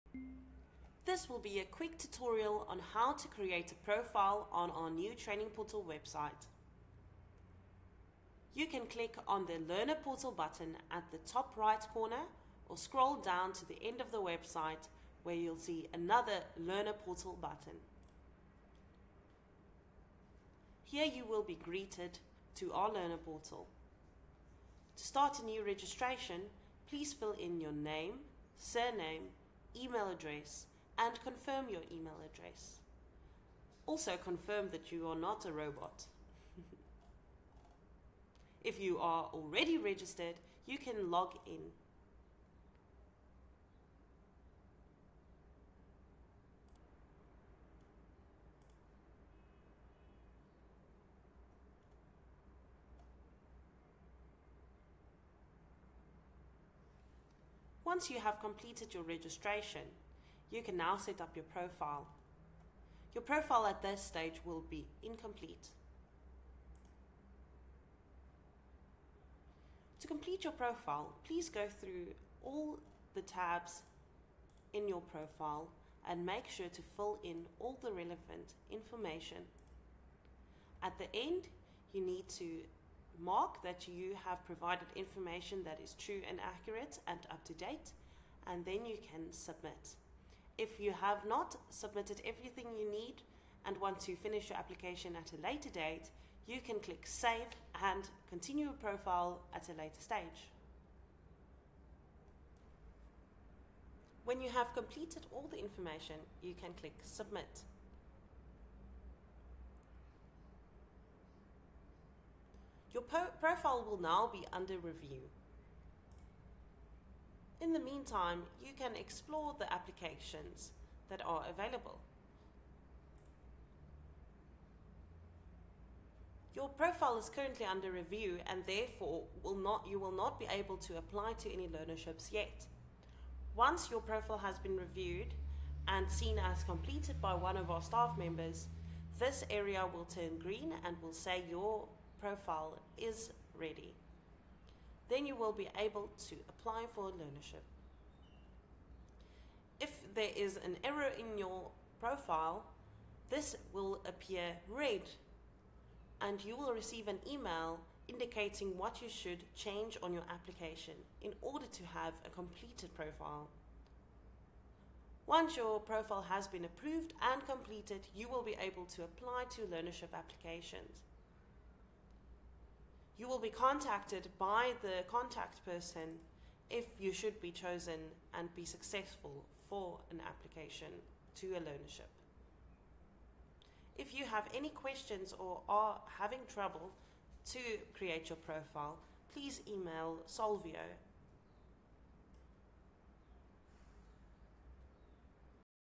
How To: Create a Learner Profile and Apply for Learnerships on our New Learner Portal
Welcome to Training Portal's step-by-step guide on how to create your learner profile and apply for learnerships using our new, user-friendly Learner Portal. In this video, we walk you through the entire process—from setting up your profile to submitting your learnership applications with ease. Whether you're a first-time applicant or a returning learner, this tutorial will help you navigate the portal confidently.